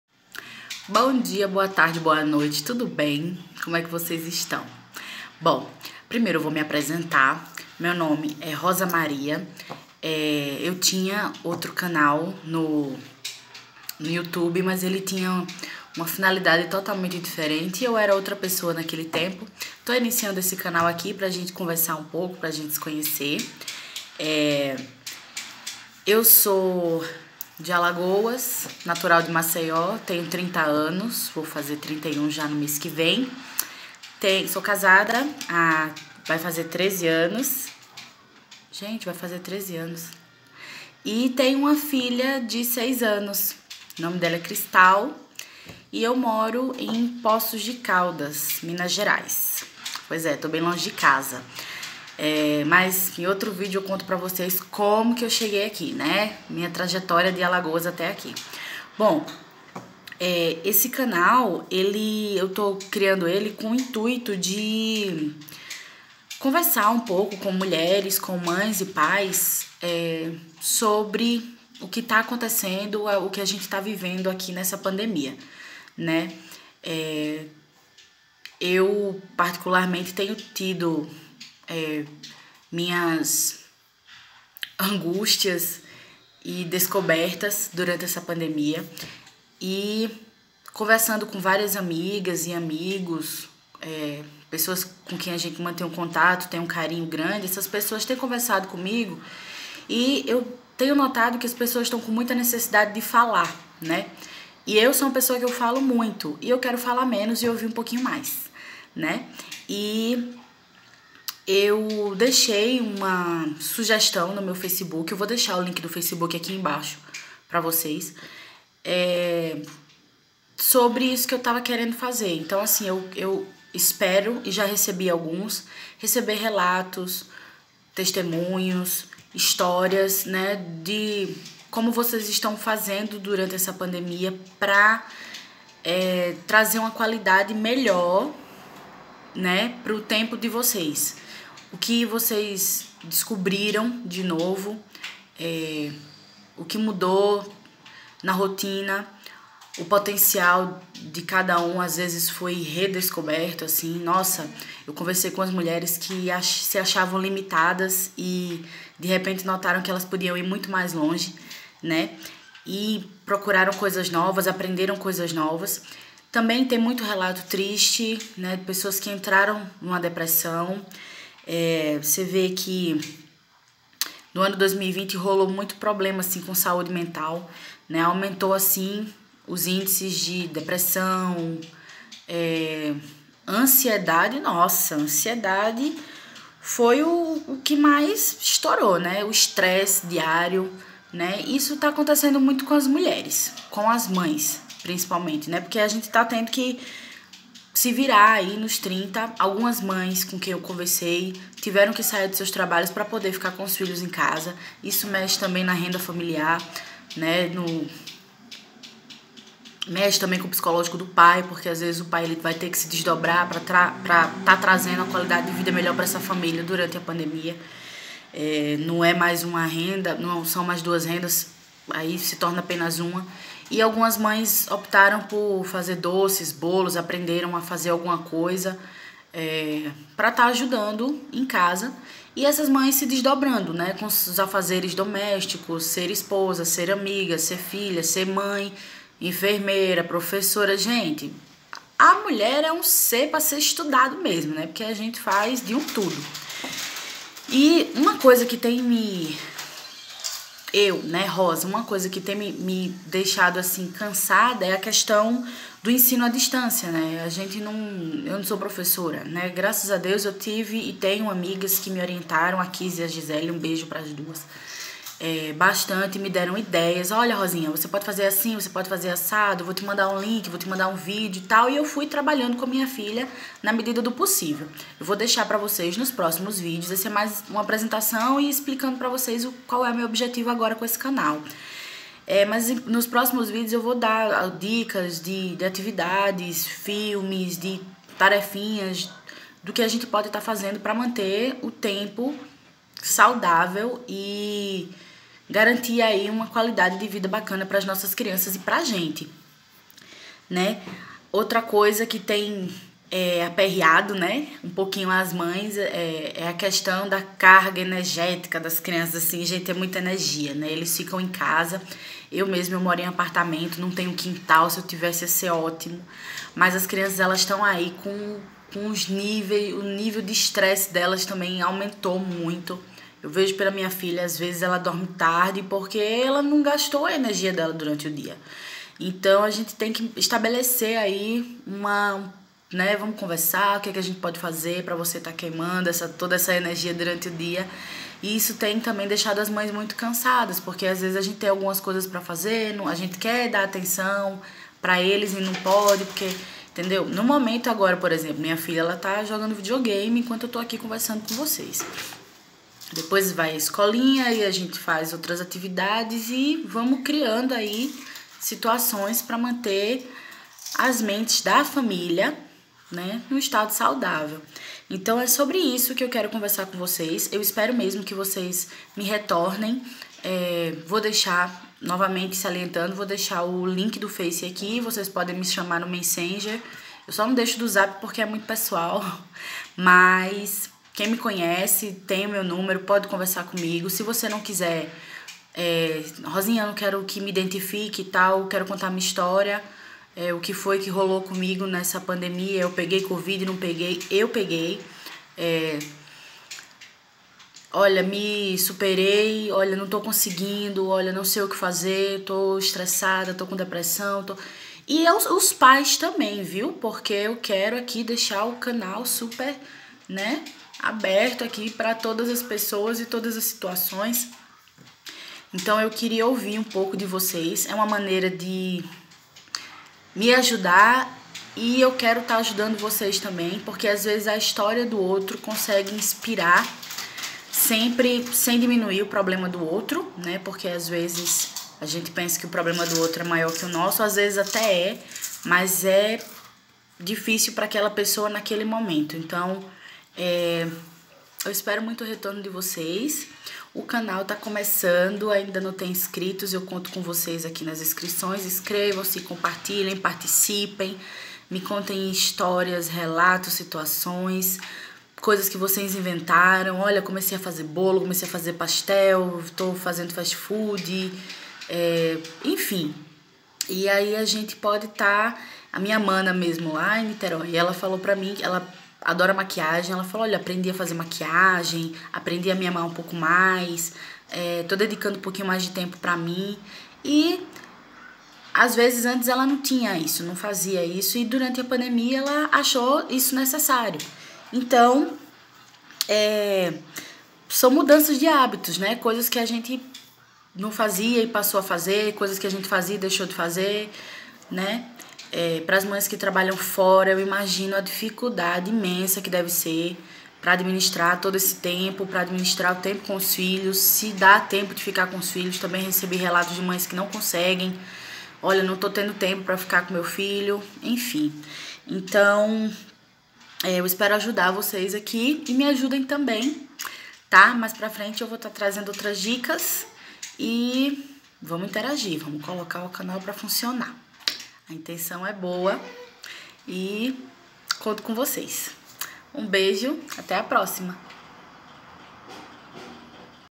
Pandemia. Iniciando o canal.
Espero que gostem da ideia e participem, vamos nos unir e ajudar umas as outras.